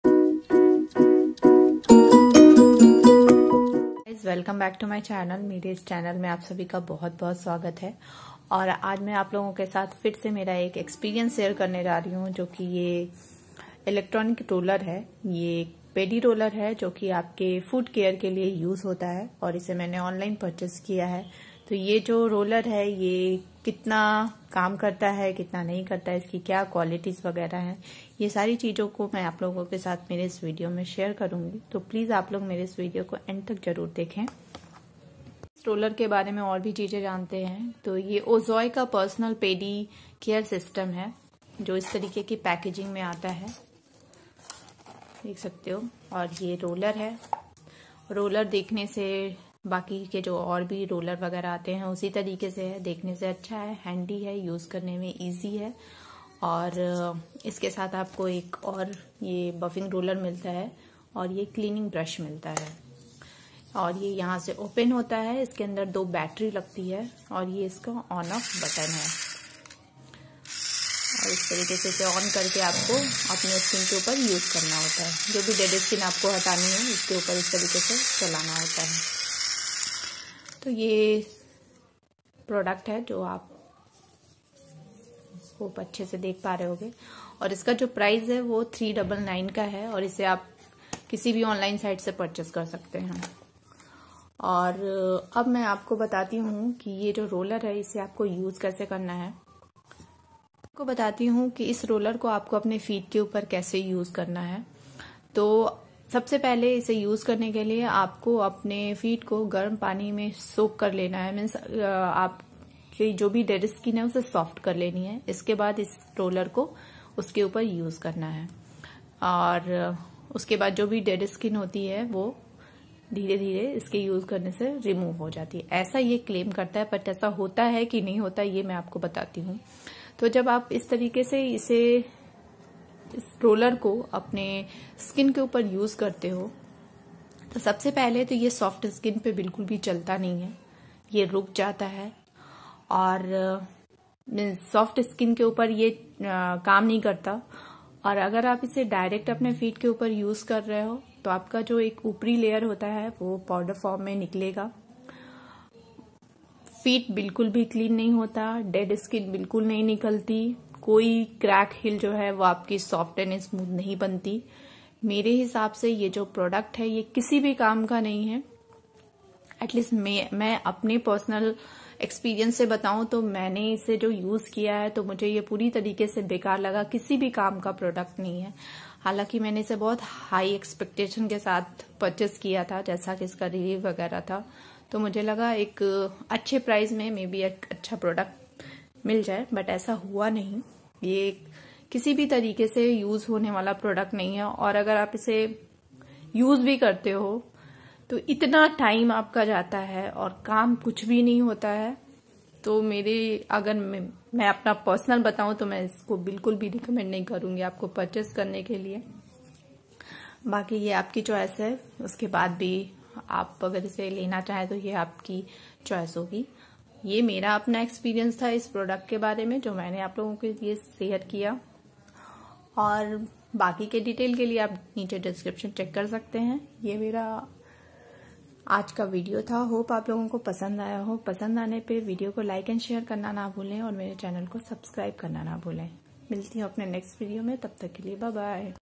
Ozoy pedicure remover | Ozoy Foot Callus Remover Review | Not Recommended
In this video I will review Ozoy Pedicure Dead Skin Callus Remover for Feet…I would not recommend this product though I bought it with great expectation. I am disappointment by its performance and result..

Isko Mat Kharido…Not worthy of the a product…

DESCRIPTION OF THE SKINPLUS MAKE BRUSHES:
* IPX7 waterproof hardened mineral surface rollers.
* Battery operated, which can be used cordless for 150 minutes on full charge. The product should not be used when it is getting charged
* Extra roller heads that spin 360 degrees for quick results on thick, calloused skin